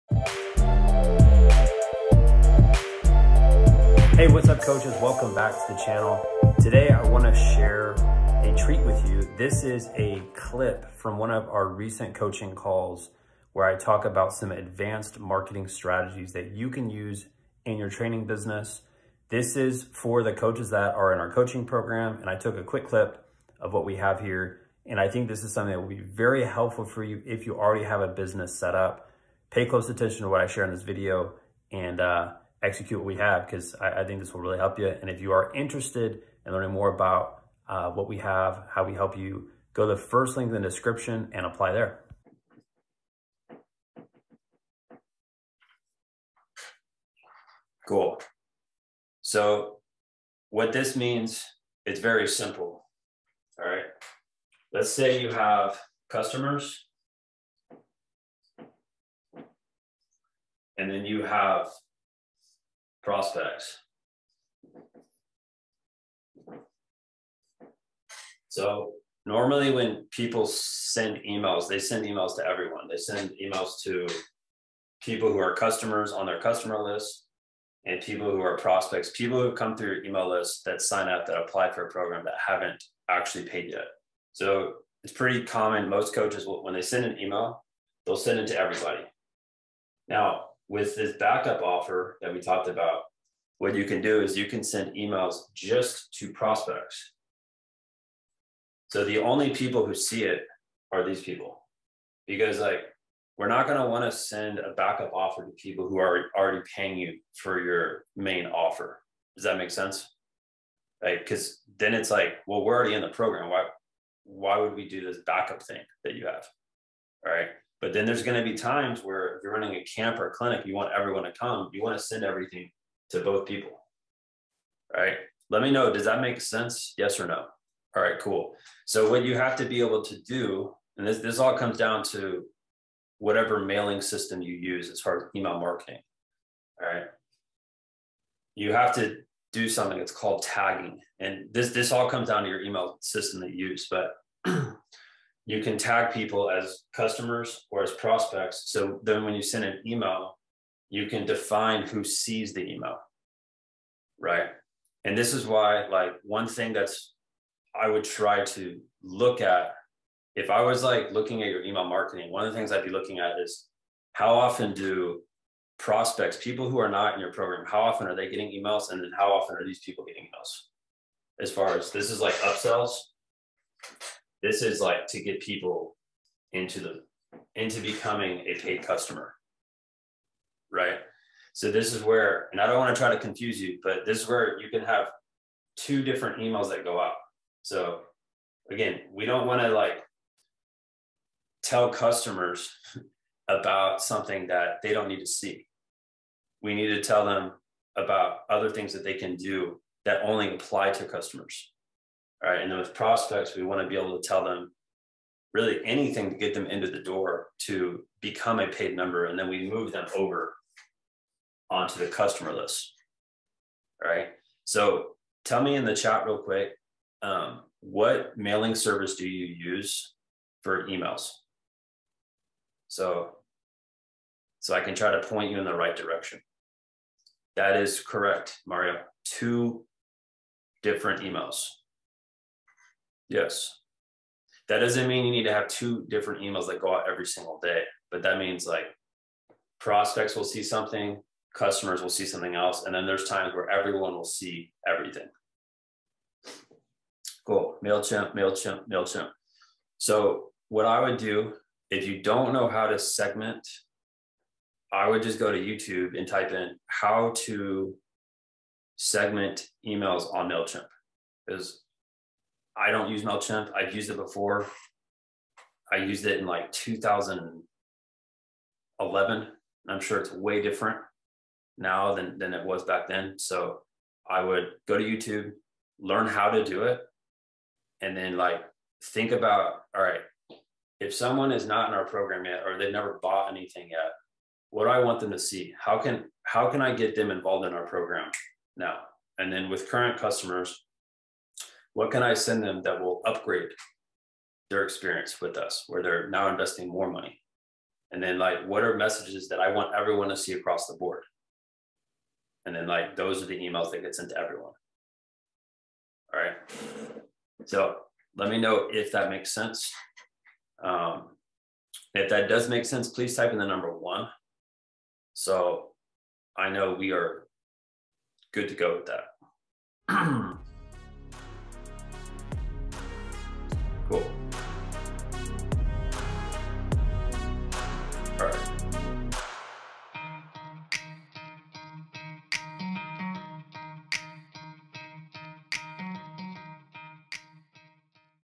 Warning: Advanced ONLINE Marketing Strategies to get CLIENTS in your SOCCER/BASKETBALL Business
In today's video, Ben shares a segment from his accelerator business coaching program that breaks down an ADVANCED marketing strategy that will help you get more clients in your basketball/soccer/sports business